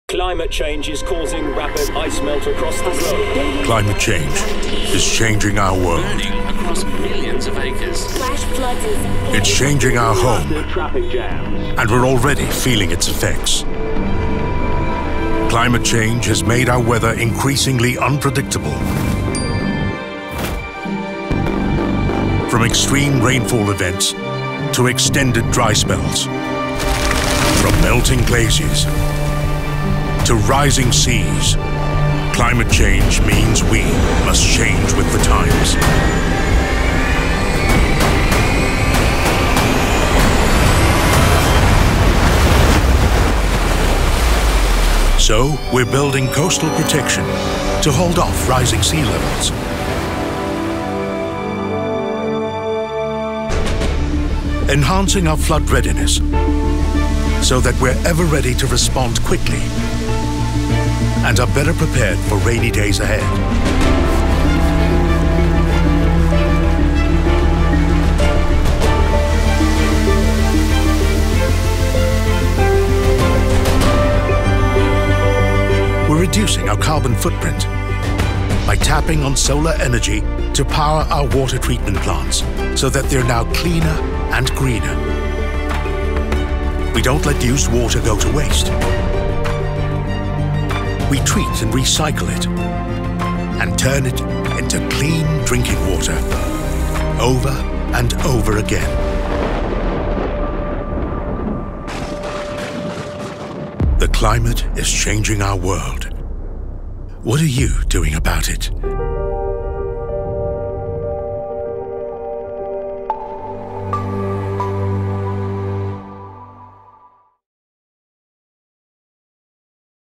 The Climate Is Changing
Climate change is changing our world – and our home. We are already seeing and feeling its effects.

But it goes beyond unpredictable weather and rising temperatures. It’s more than the extreme rainfalls and extended dry spells we have experienced. Climate change poses an immense threat to our water sustainability.

Watch our film to see the far-reaching effects of climate change, and what we can do in our fight against it.